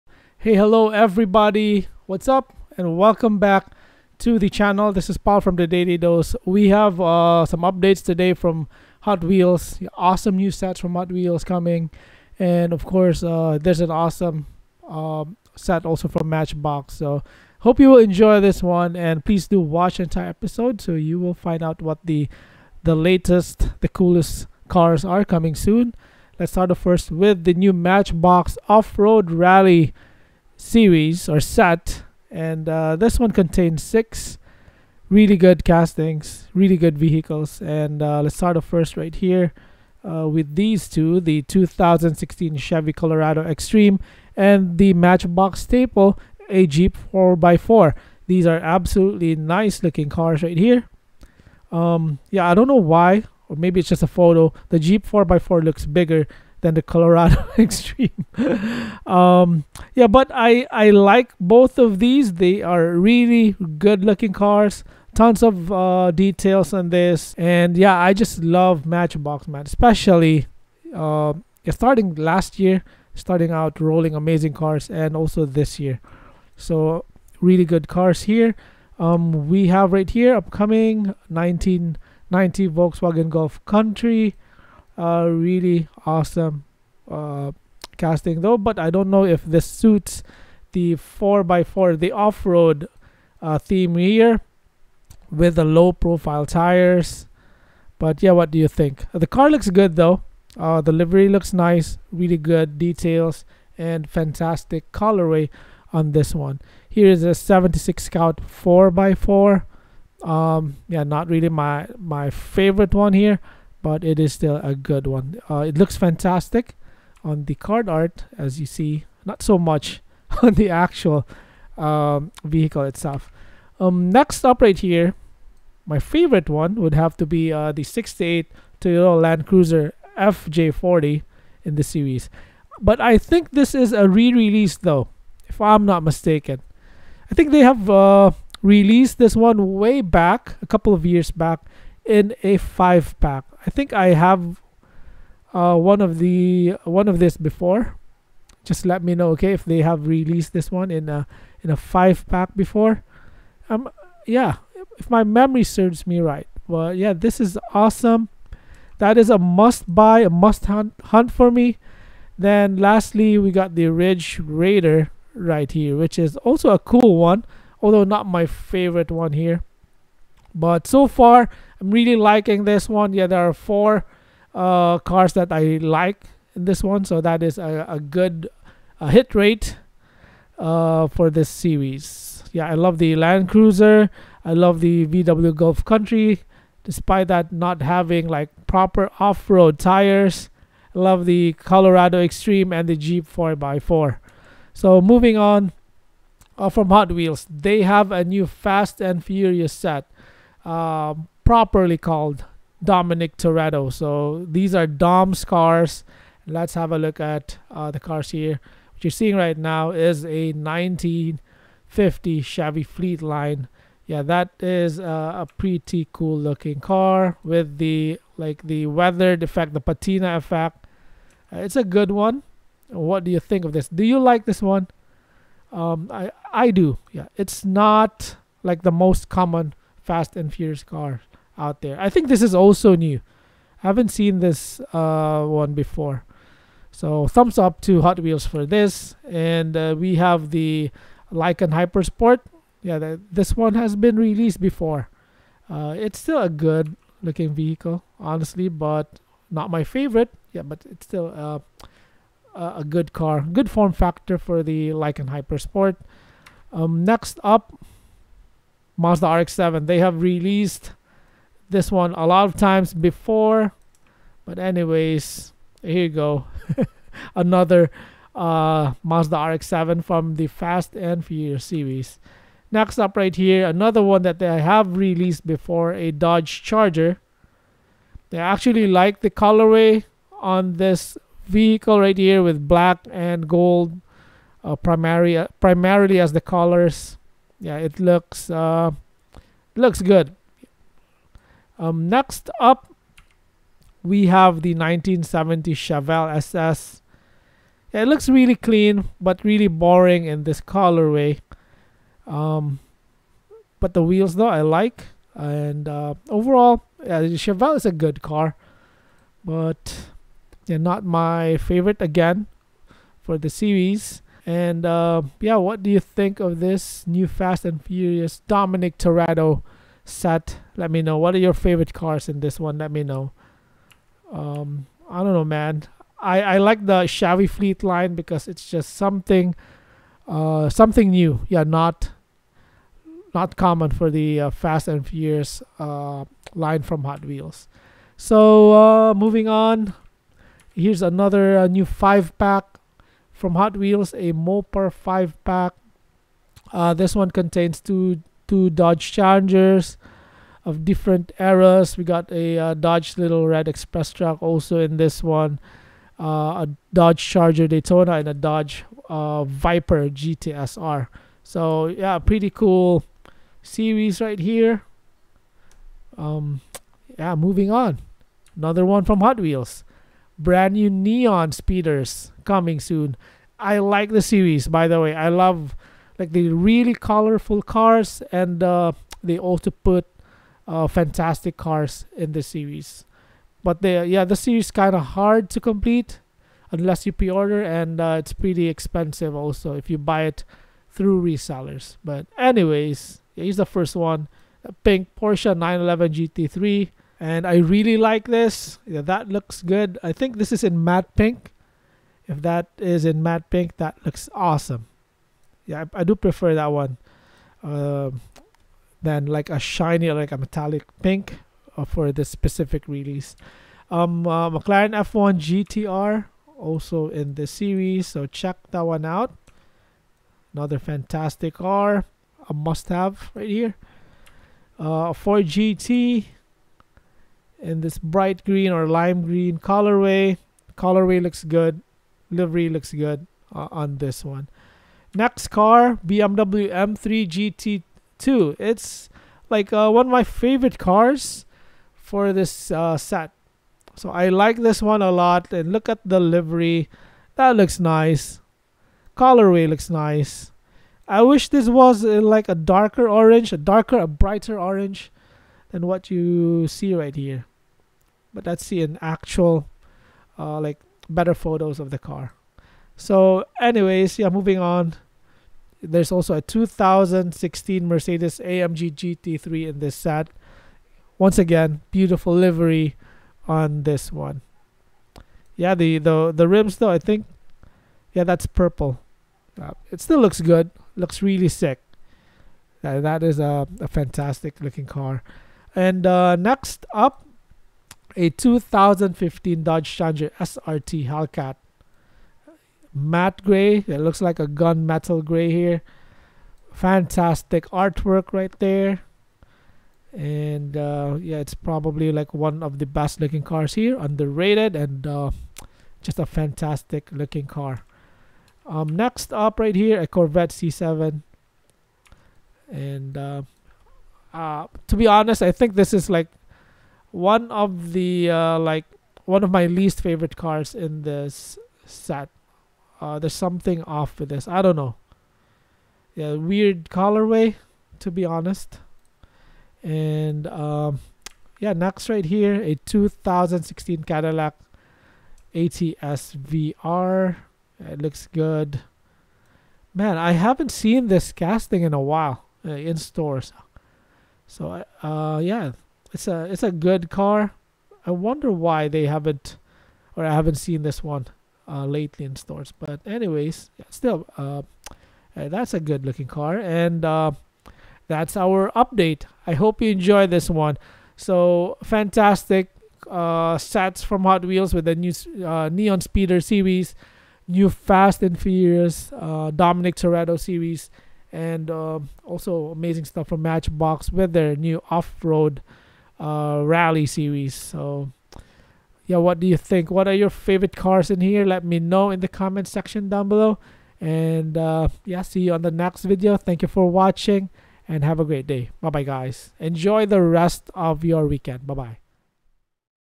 New Hot Wheels Fast and Furious Dominic Toretto Set, New Neon Speeders, Matchbox Off road Rally
Thank you T-Hunted and Thinkdiecast for the photos :)